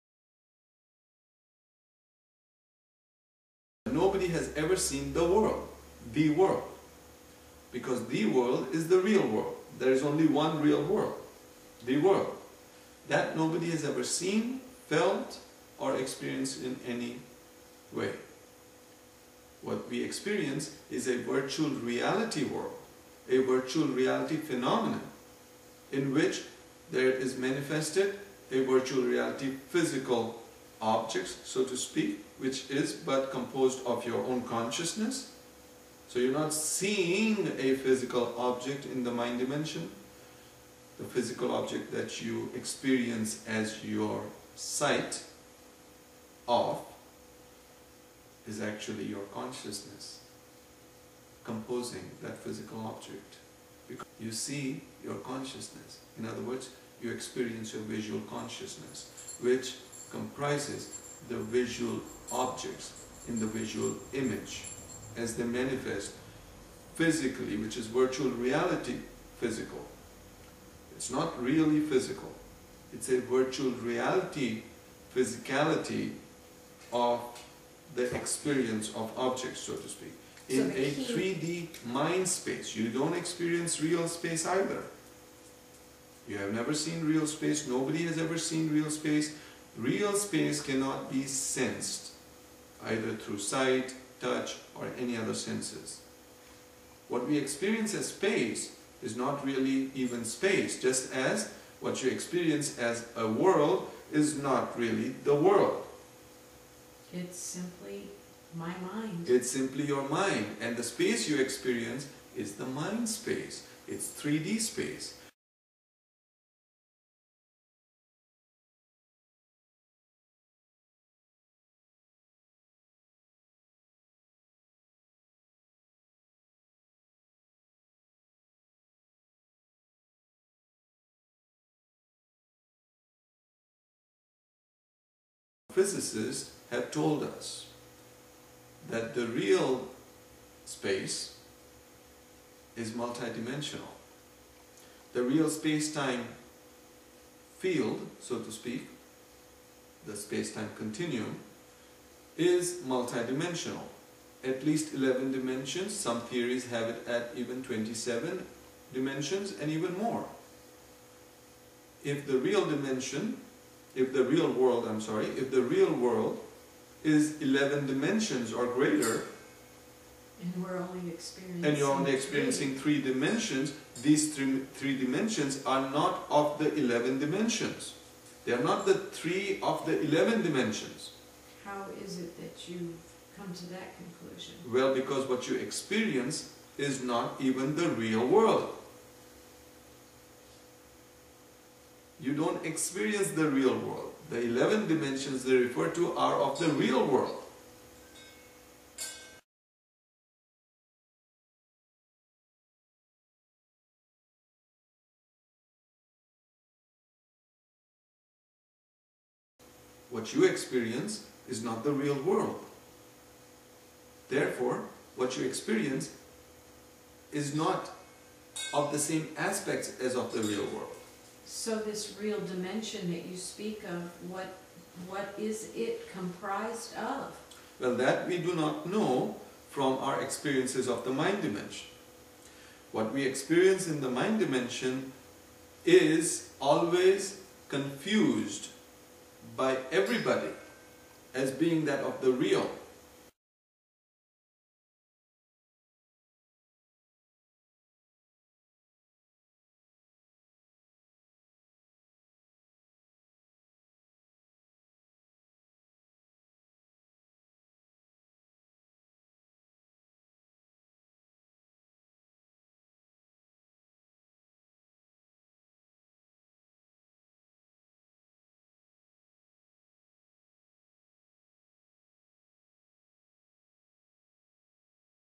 3D Mind-Space - It's Not Real Space!
TO LEARN THAT THE REAL WORLD, INCUDING YOUR REAL BODY, IS INVISIBLE. NOBODY EXPERIENCES THE "WORLD", THE ONE AND ONLY REAL "WORLD" AS SUCH.

Neither you, nor any entity in the universe, has ever seen or experienced any real thing in the Real World.

In a stepwise systematic scientific manner, Ashoi elucidates the reality of the world (which is not really the world) a human being (you) experiences through sensory perceptions. In vision perception, a person experiences a 3-D virtual-reality "visual-image" that the person's own brain-mind system generates and manifests in the Mind Dimension. The 3-D visual-image is but only a virtual-reality image -- it is not the real world, however, it is made to appear world-like in experience.

What you experience at any given moment, for instance at this moment, is but a virtual-reality sensory mechanism comprising your own personal consciousness. Your brain-mind system is a neuroelectrical sensory data-processor and a virtual-reality world generator.

Hence, remember that you do not actually experience the world (the real world, which, by the way, is the only world there can be) as such. You experience only a sensory world-like presentation, which comprises your own mind in the Mind Dimension.

Ashoi brings us his profound insights of reality in ways no other has before.